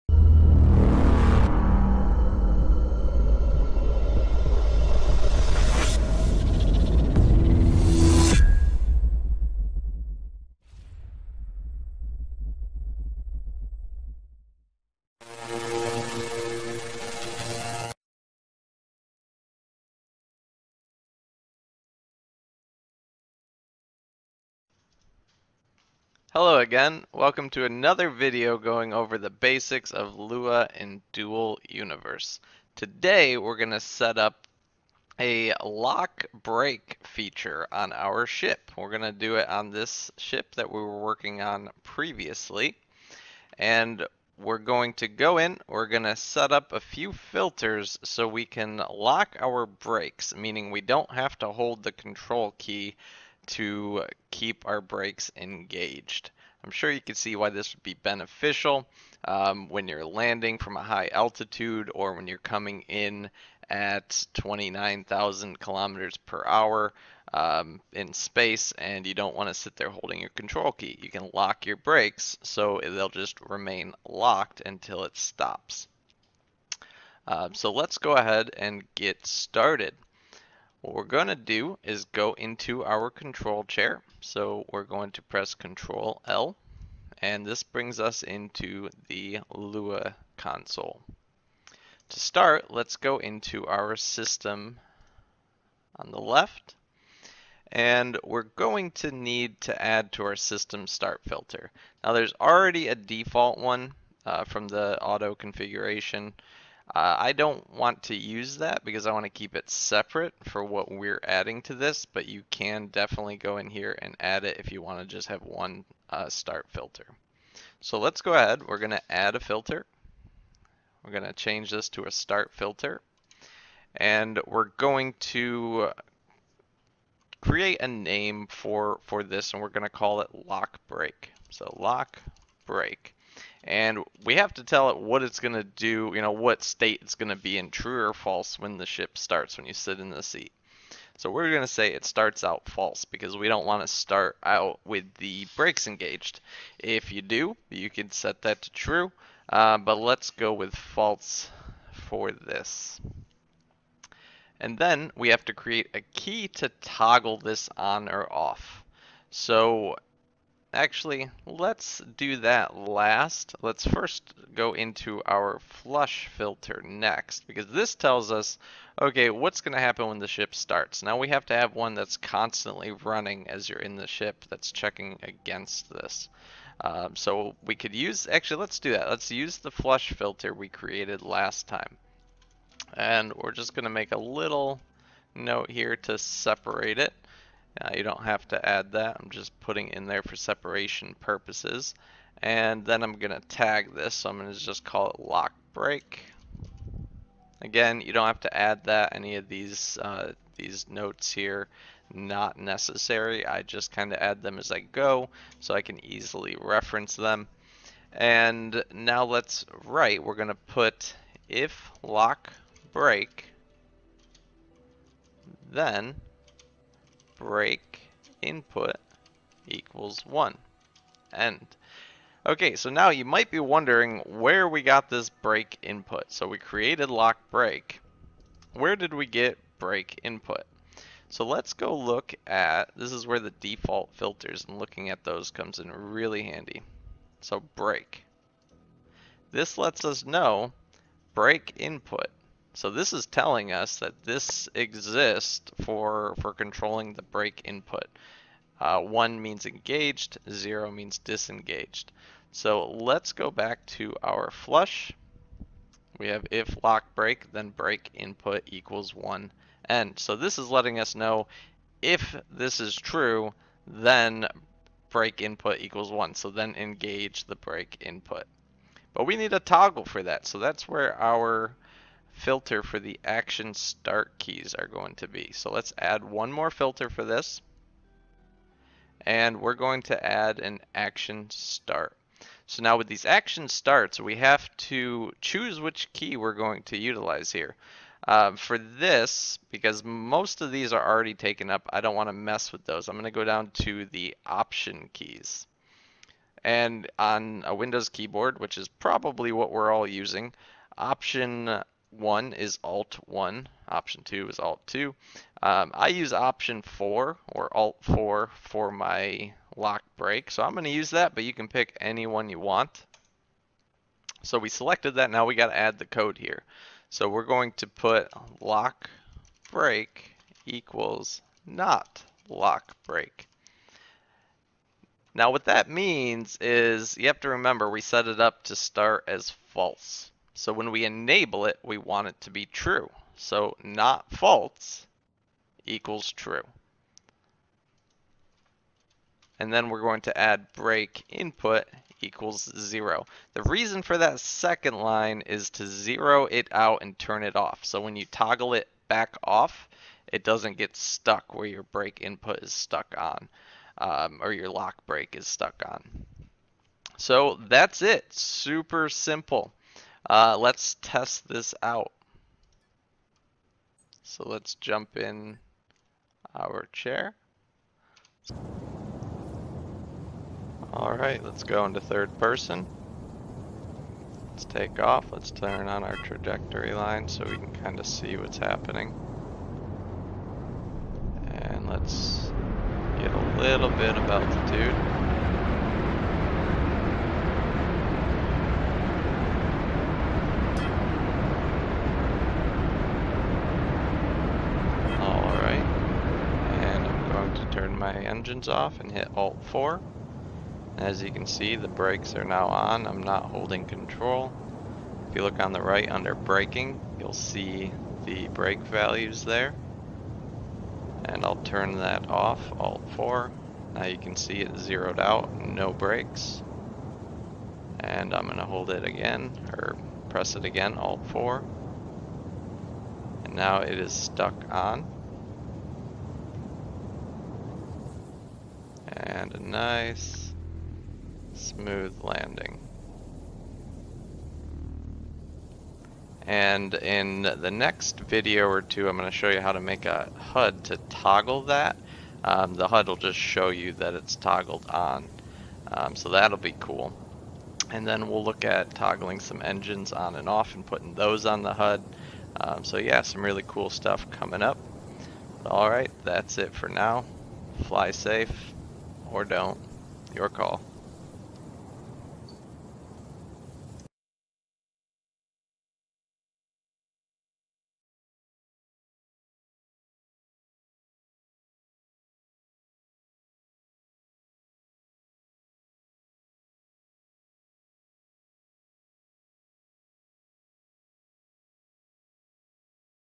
Lua Tutorial Brake Lock
In this episode we'll setup a key to lock our brakes on, instead of using Cruise Control or having to hold down Control.